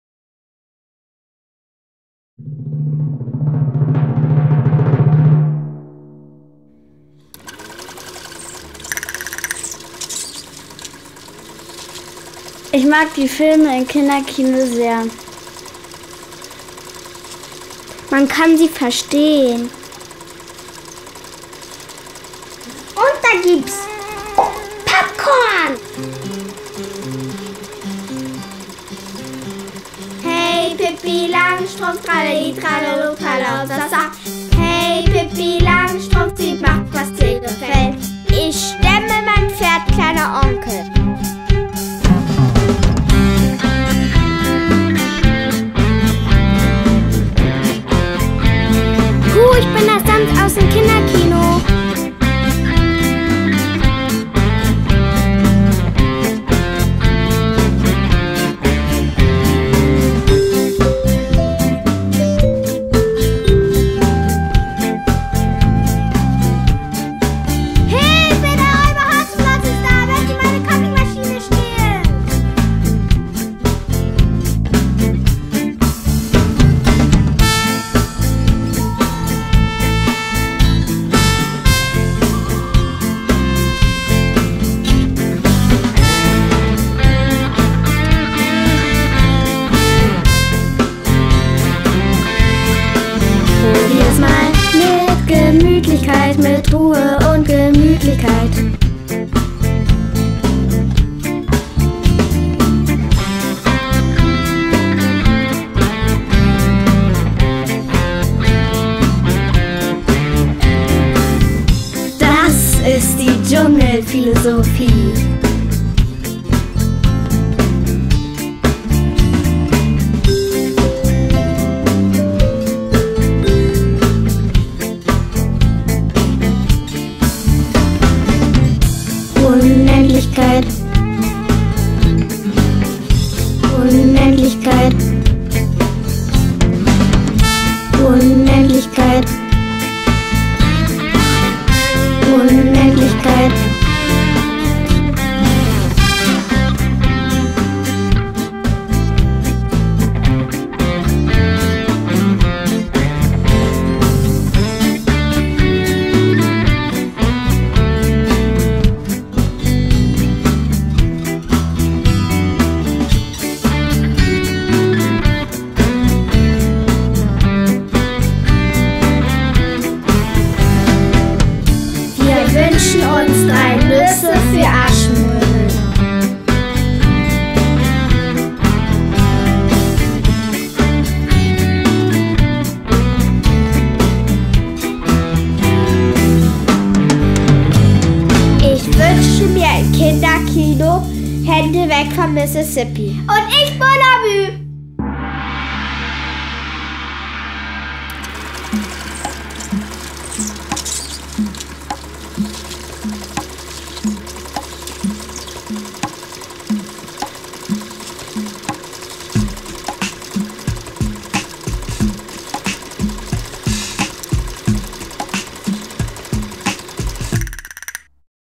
30 Jahre Kinderfilmring Hamburg
Ein Film von: Nicole, Lena, Rafaela, Hanna, Katharina, Osamuyi,Fenja 6 Mintje

2013 in den Ateliers der Honigfabrik Wilhelmsburg